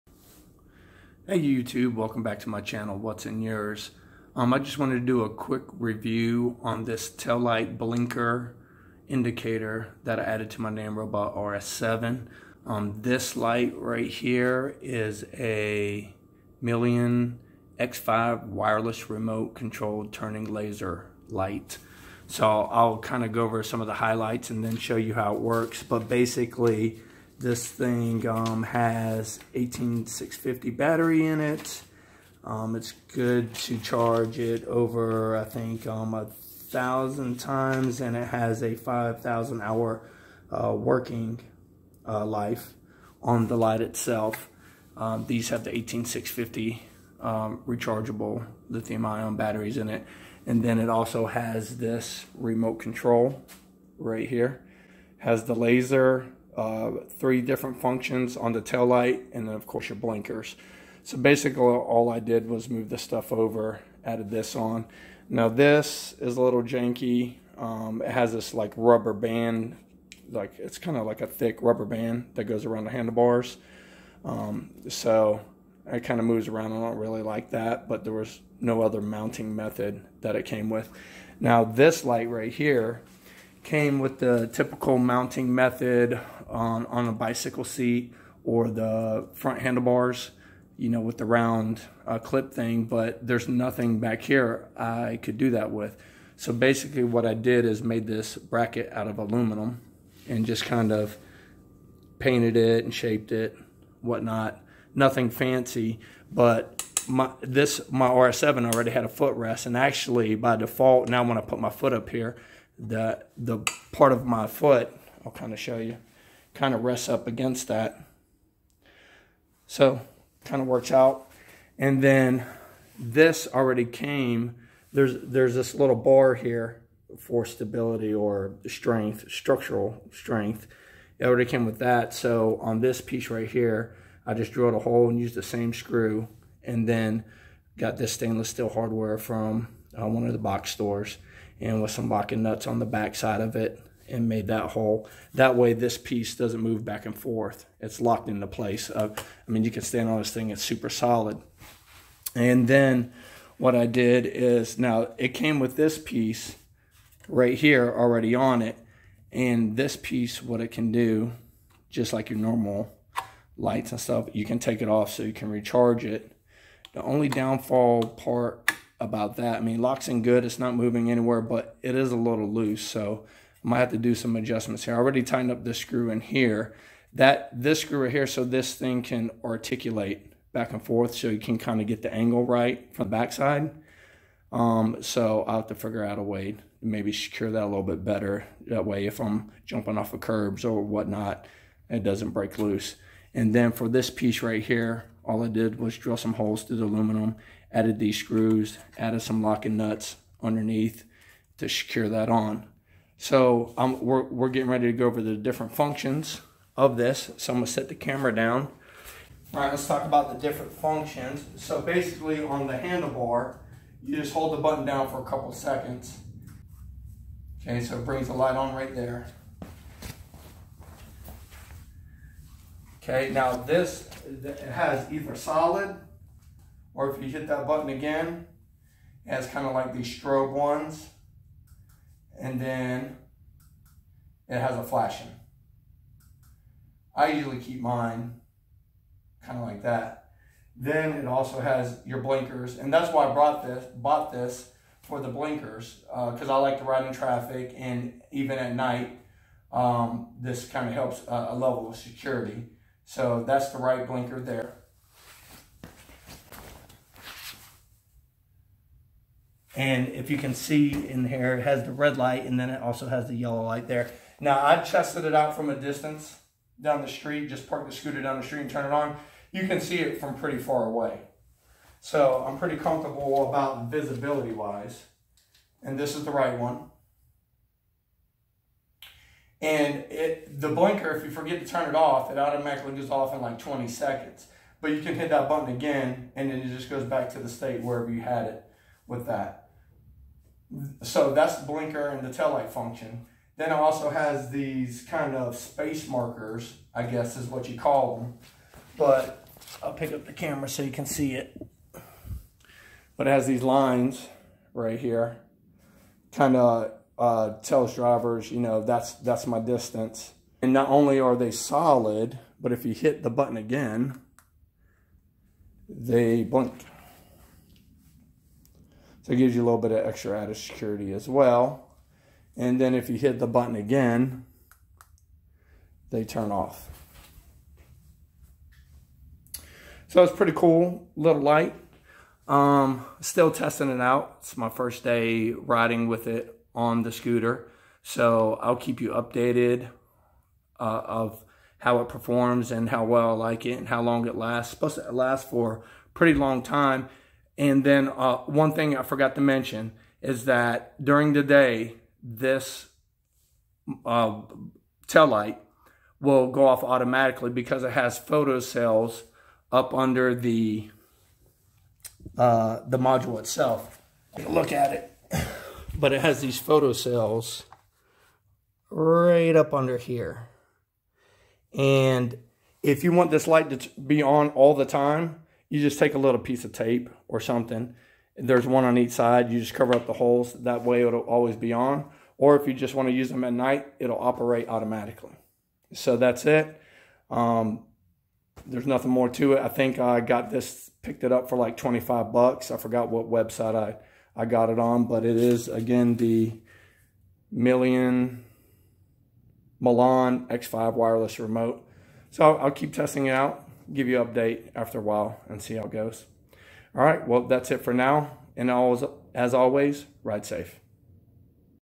How I added turn signals to my electric scooter. motorized scooter. nanrobot.
In this video I added turning signals to my electric scooter. Some modification were made, because I had to add a bracket. Outside of that is pretty much plug and play.
Made video with Win Movie Maker, you can split,rotate add watermark and subtitle etc. You can download